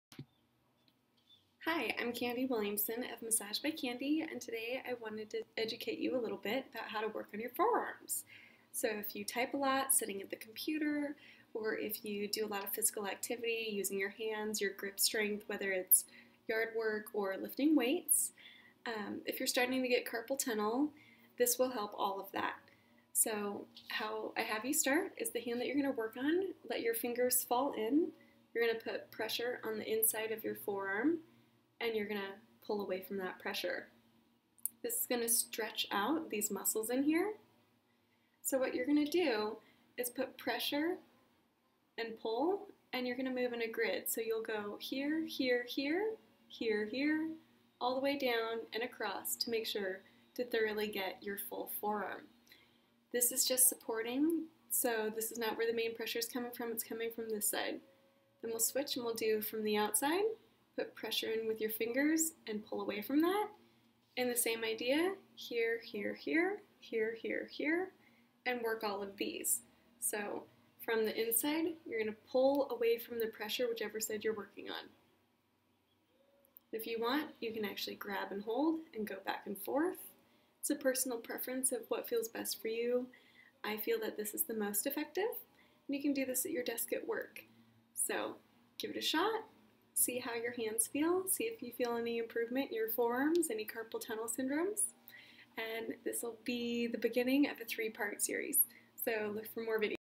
Forearm Soft Tissue Release
If you suffer from Carpal Tunnel Syndrome, Lift Heavy Weights and feel pain or weakness in your grip strength,  spend a lot of time typing or working on your computer these stretches will help alleviate pain and discomfort.  This is one of there self care videos specific to your forearms and hands.